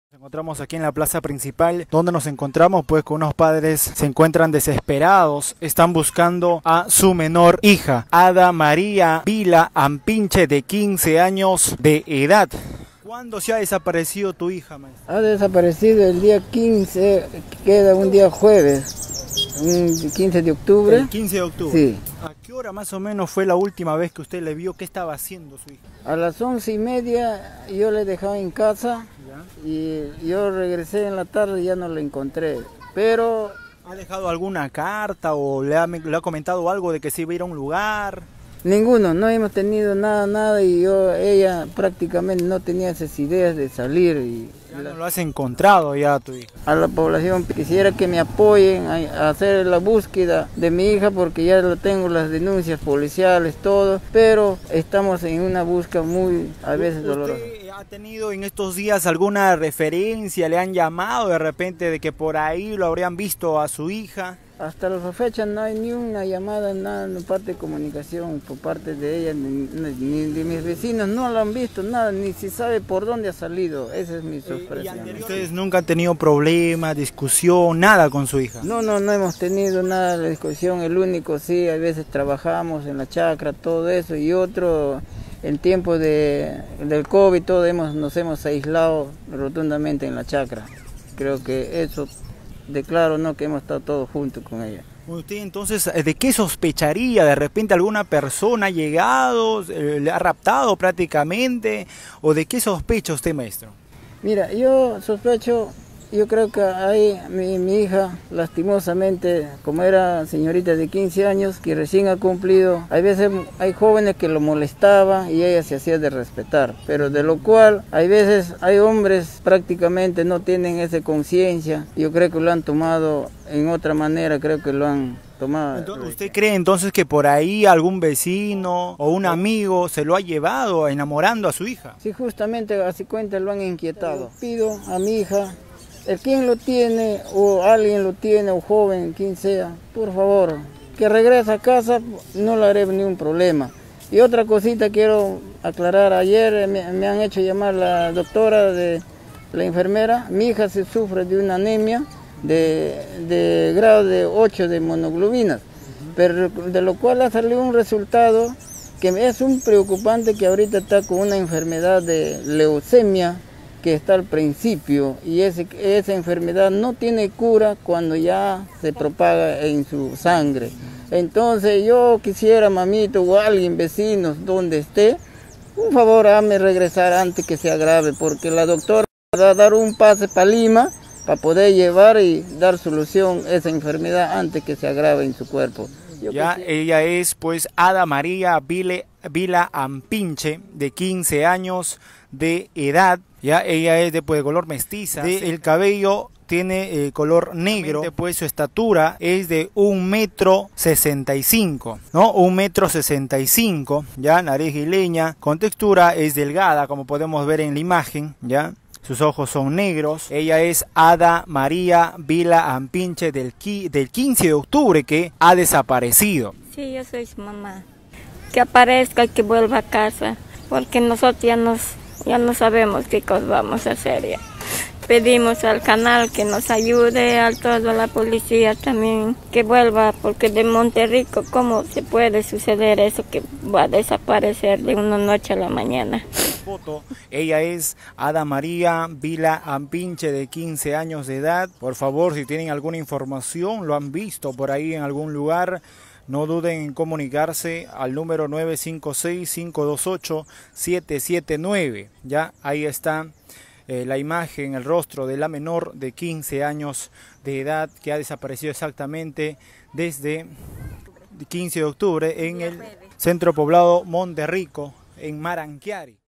SATIPO: Padres buscan a hija desaparecida | KDNA NOTICIAS
Adolescente desapareció el 15 de octubre en el C.P Monte Rico - Marankiari. Sus padres Maximo llanco y Nely Paredes se encuentran Desesperados, Mencionan que se habría fugado con su enamorado quien la inquietó.